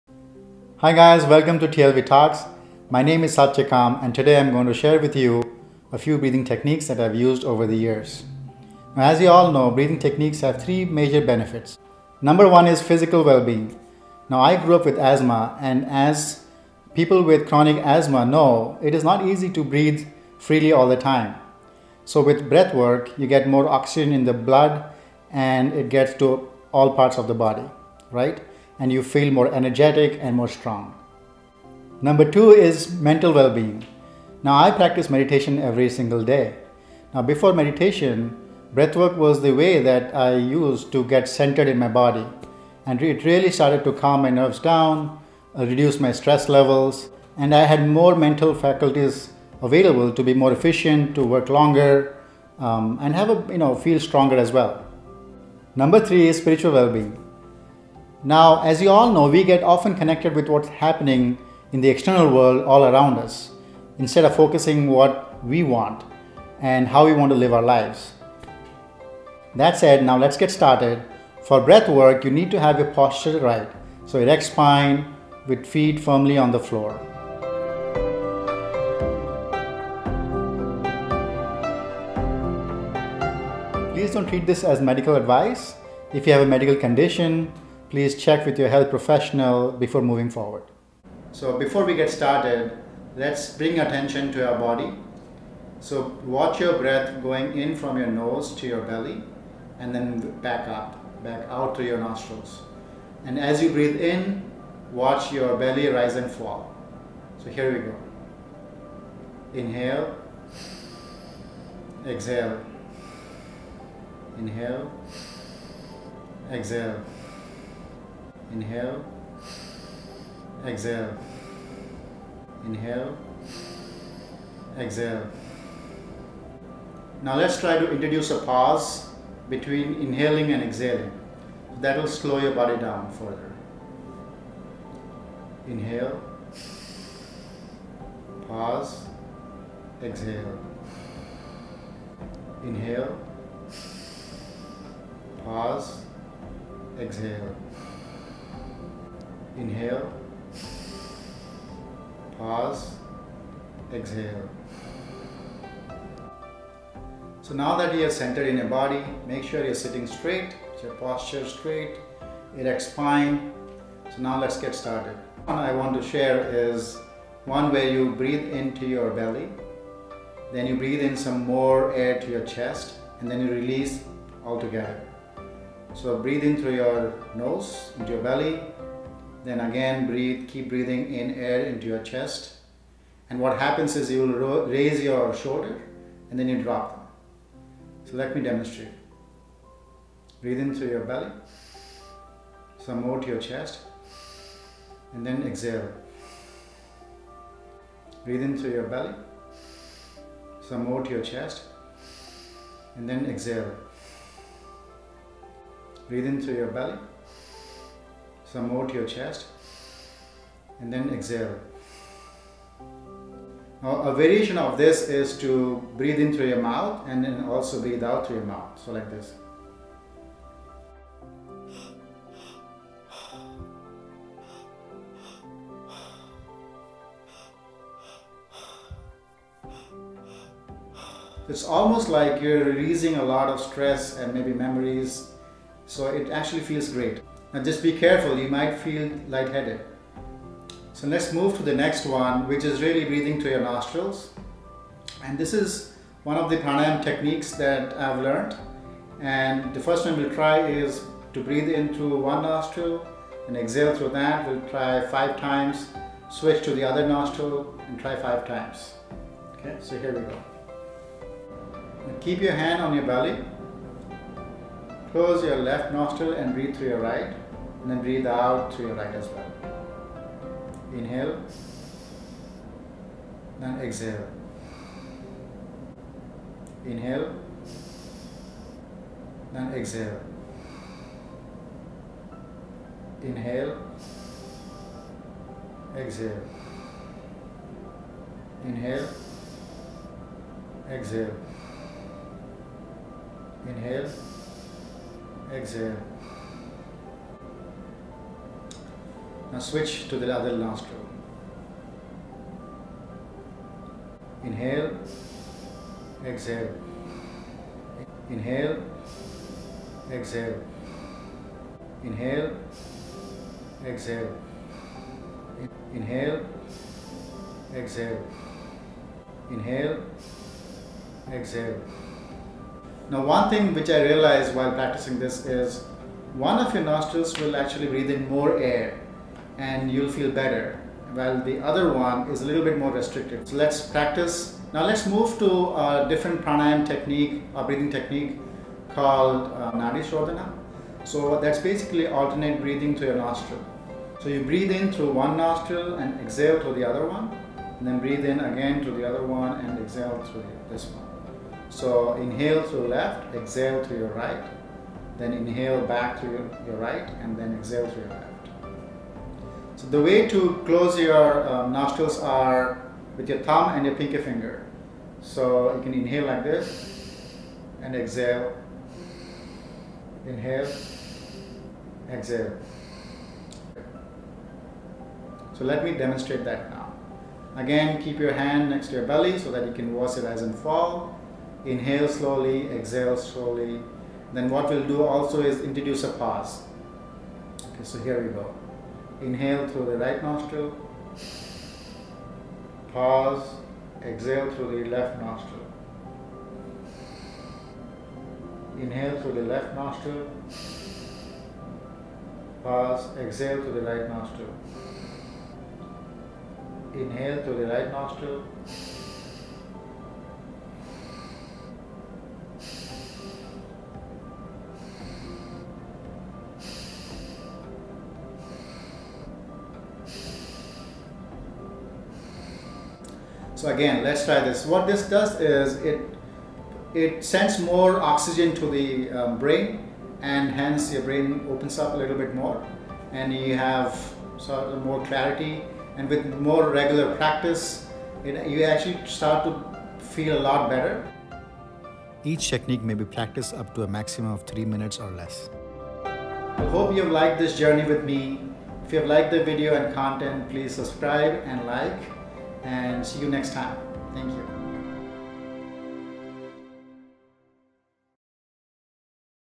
Simple Breathing Techniques To Relieve Stress That May Change Your Life
Breathing Techniques demonstrated by Satyakam Das
1. Regular Breathing With Pause
2. Emotional Healing Breathing
3. Single Nostril Breathing
4. Alternate Nostril Breathing - NadhiSodhana

These breathing techniques help relieve stress and anxiety, and help you breathe better.

Thanks,
Rashmi

Instagram